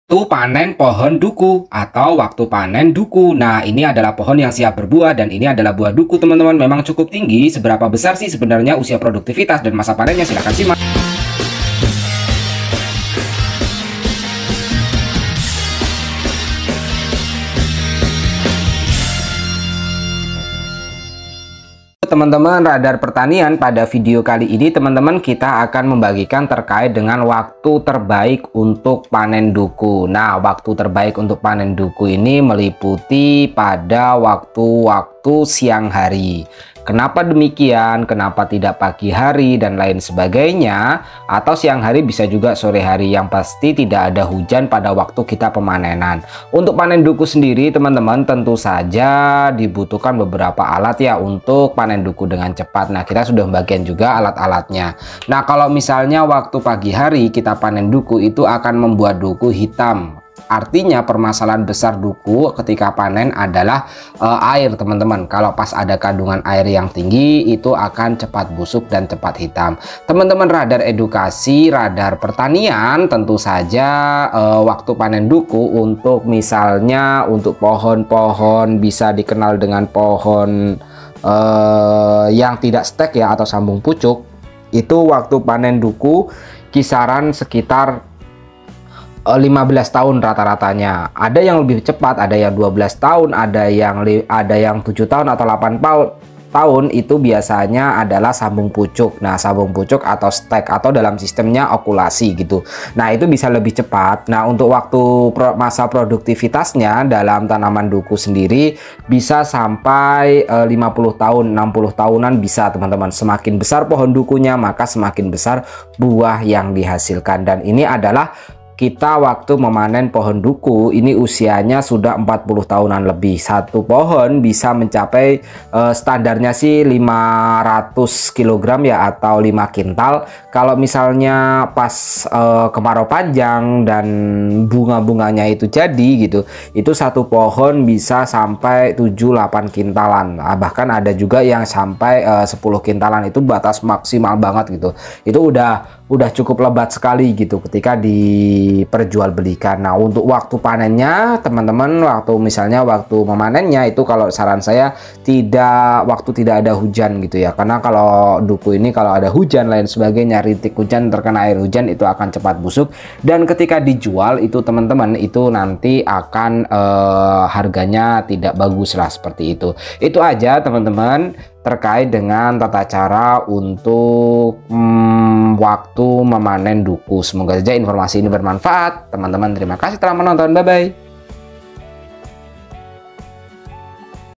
Waktu Panen Duku Yang Tepat
Waktu panen buah duku yang tepat akan menghasilkan buah yang awet dan kualitas bagus sehingga jika dijual akan mendapatkan harga yang mahal. Namun demikian sangat disayangkan bahwa petani duku banyak yang belum mengetahui cara memanen duku yang tepat, baik secara waktu maupun teknik pemanenannya.

Berikut ini merupakan cara yang dapat dilakukan untuk memanen buah duku agar berlangsung cepat dan bisa menghasilkan buah dengan kualitas yang baik. Semoga video @RadarPertanian dapat memberikan inspirasi kepada temen-temen semua dalam melakukan panen buah duku.